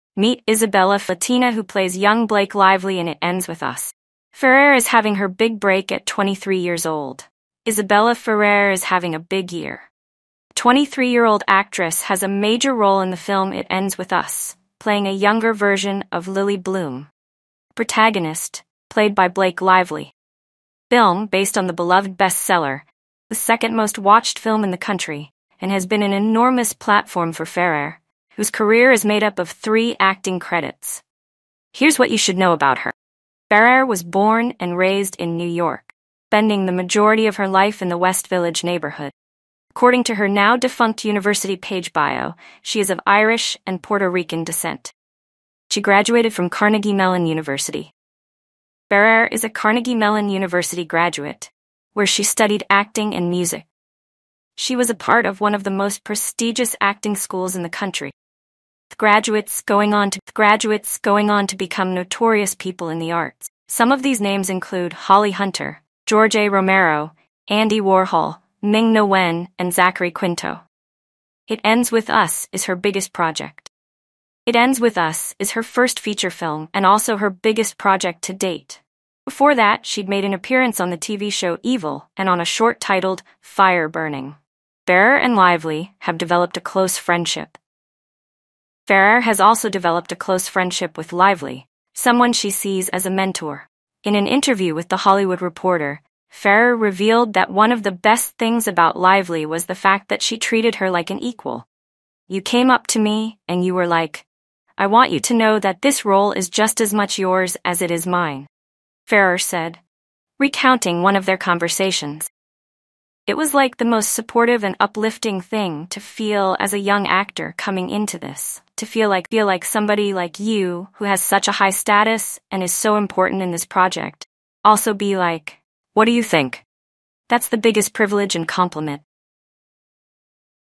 Meet Isabela Ferrer, the Latina who plays young Blake Lively in It Ends With Us
Isabela Ferrer, a 23-year-old Latina actress, has landed a major role in the film "It Ends With Us," playing a younger version of Lily Bloom, the protagonist played by Blake Lively. The film, based on the best-selling book, is the second most watched in the country and has been a significant platform for Ferrer, who has three acting credits. Born and raised in New York, Ferrer is of Irish and Puerto Rican descent and graduated from Carnegie Mellon University, where she studied acting and music. She has also appeared on the TV show "Evil" and a short titled "Fire Burning." Ferrer sees Lively as a mentor and feels honored to be part of the project.